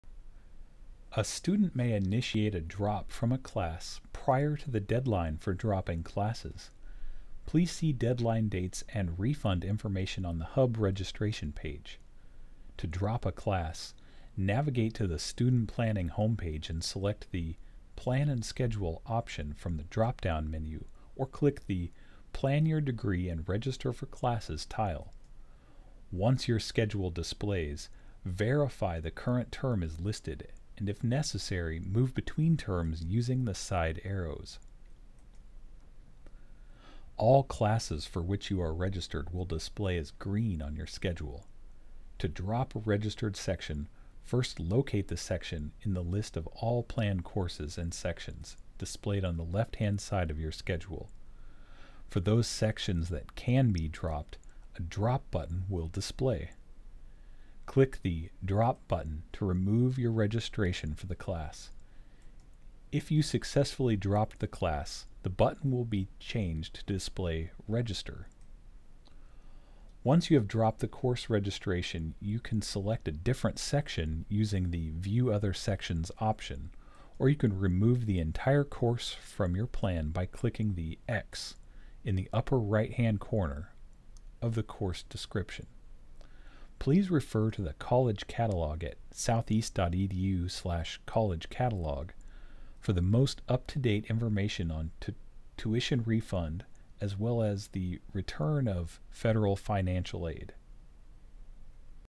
Tutorial #9: How to Drop a section
A student may initiate a drop from a class prior to the deadline for dropping classes.  Please see deadline dates and refund information on The Hub Registration page.

To drop a class, navigate to the Student Planning home page and select the “Plan & Schedule” option from the drop down menu or click the “Plan your Degree & Register for Classes” tile. Once your schedule displays, verify the current term is listed and if necessary, move between terms using the side arrows.

All classes for which you are registered will display as green on your schedule.  To drop a registered section, first locate the section in the list of all planned courses and sections displayed on the left hand side of your schedule.  For those sections that can be dropped, a “Drop” button will display.  Click the “Drop” button to remove your registration for the class.  If you successfully dropped the class, the button will change to display “Register”.

Once you have dropped the course registration you can select a different section using the “View other sections” option or you can remove the entire course from your plan by clicking the “x” in the upper right hand corner of the course description.

Please refer to the College Catalog at southeast.edu/collegecatalog/ for the most up to date information on a tuition refund as well as the return of Federal Financial Aid.